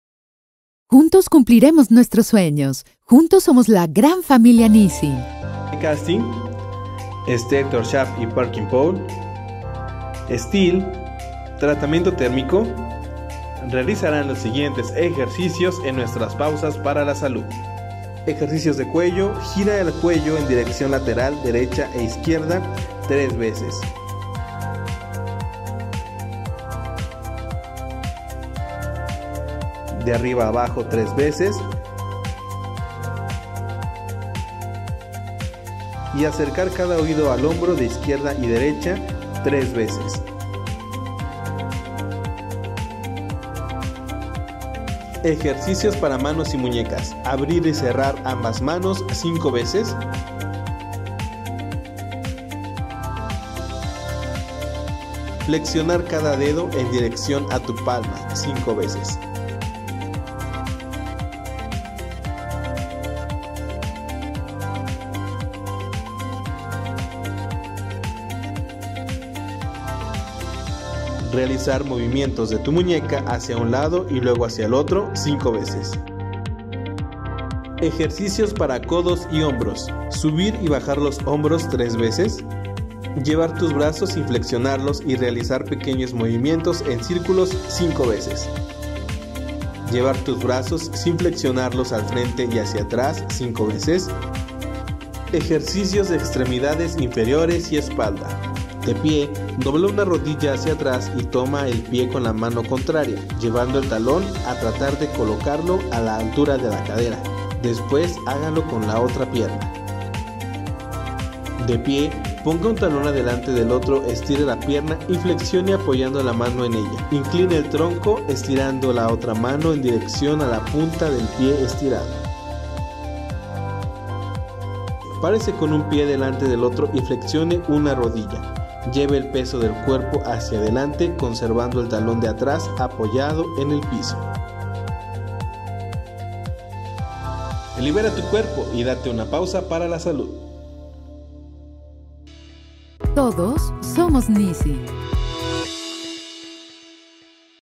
Pausas a la salud FORJA DC STS PP STEEL
Practica tu Pausa para la Salud  ÁREAS FORJA DC STS PP STEEL

Recuerda los horarios
1er Turno 09:55 am
2do Turno 05:55 pm
3er Turno 01:55 pm

Libera tu cuerpo y dale una pausa a la salud.